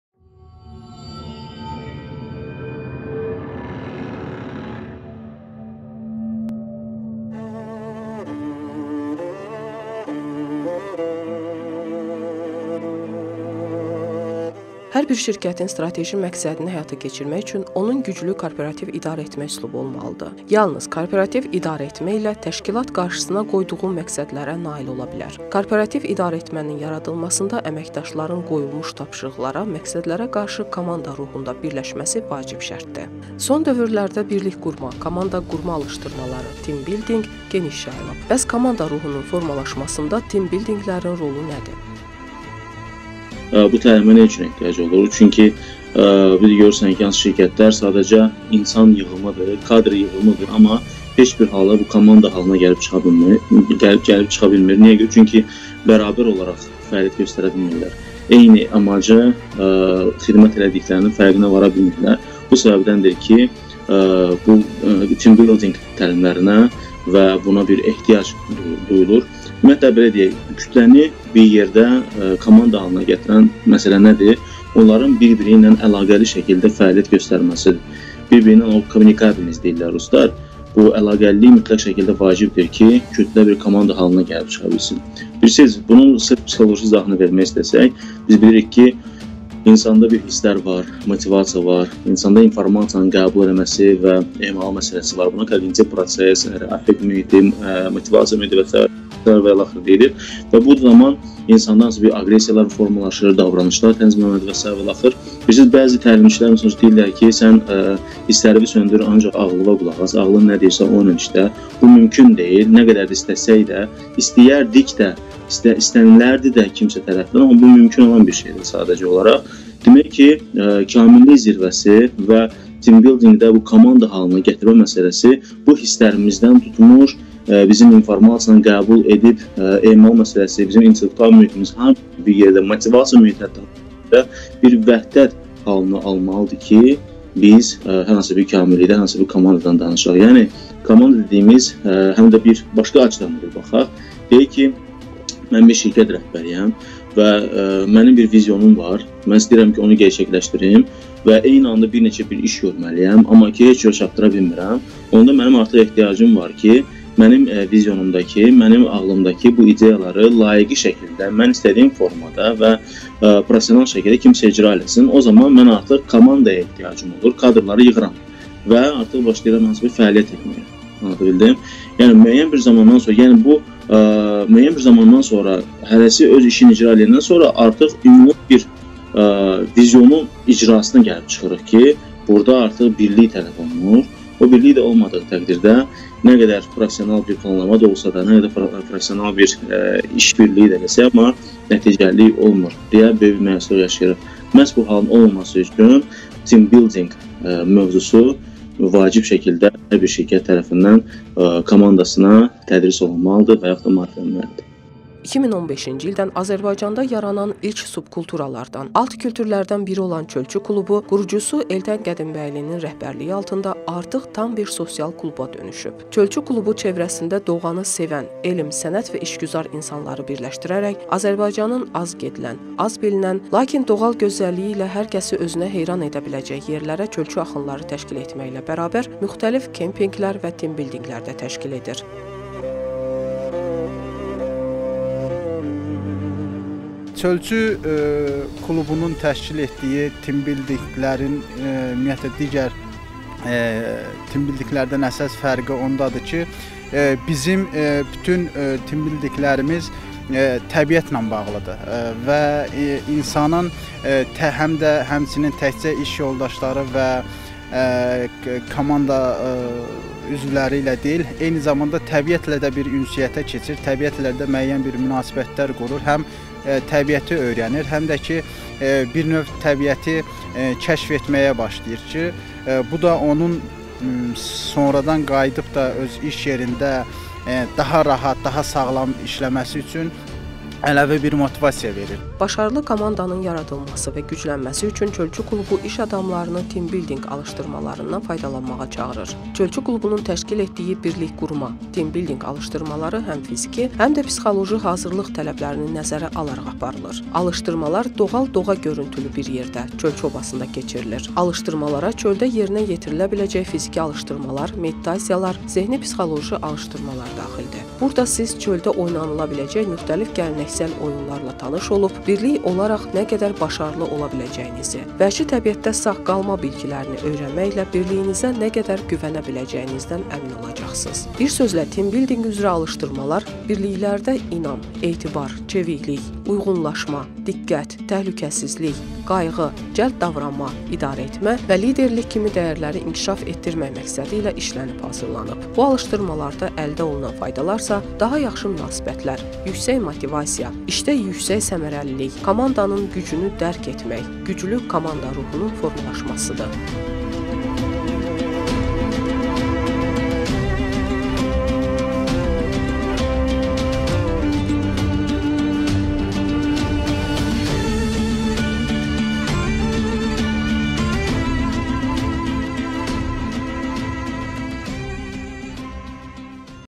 BİRLİKQURMA ALIŞDIRMALARININ FAYDALARI NƏDİR?
“Çölçü klubu”  iş adamlarını “Teambuilding” alışdırmalarından  faydalanmağa çağırır